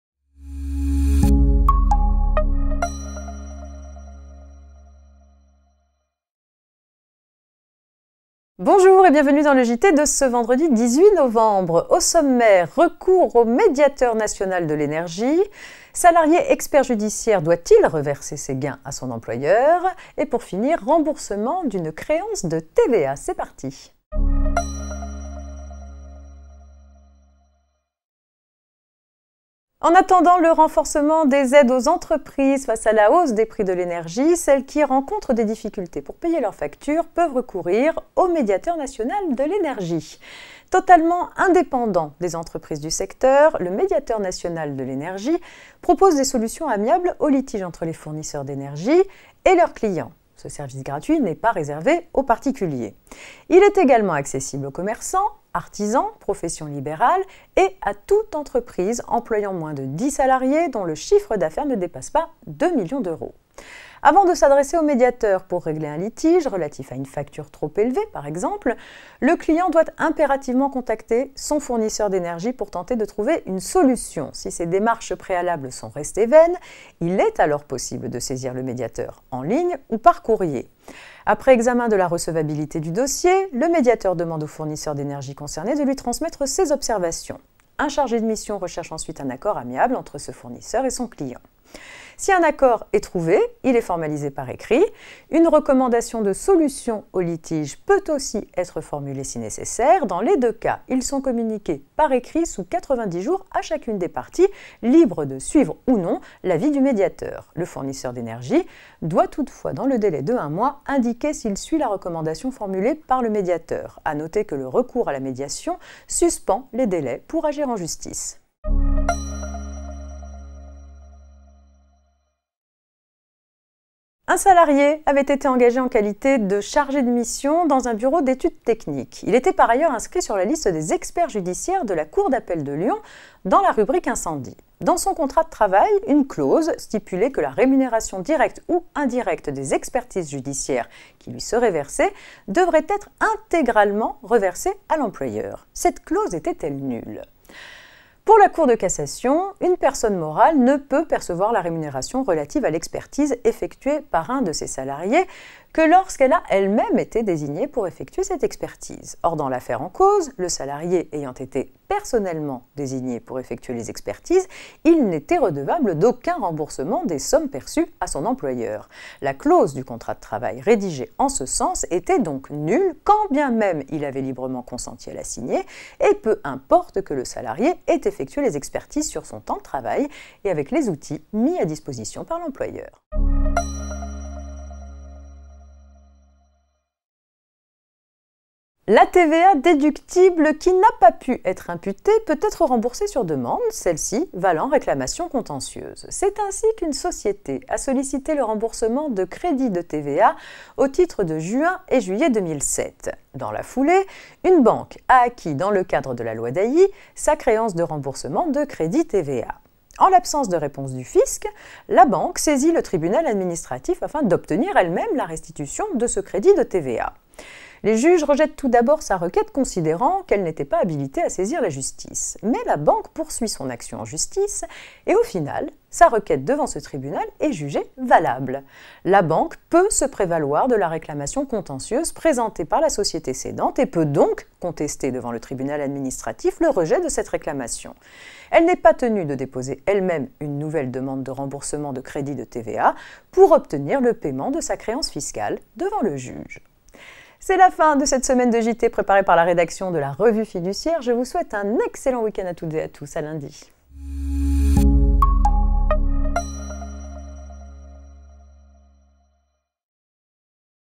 Médiateur national de l'énergie, Salarié expert judiciaire, Créance de TVA - JT du 18/11/22
À la une du JT d’actualité juridique de La Revue Fiduciaire de ce vendredi 18 novembre : Recours au médiateur national de l'énergie, Salarié expert judiciaire : doit-il reverser ses gains à son employeur ?, Remboursement d'une créance de TVA

- Cass. soc. 26 octobre 2022, n° 20-17105 FSB
- CE 22 juillet 2022, n° 451251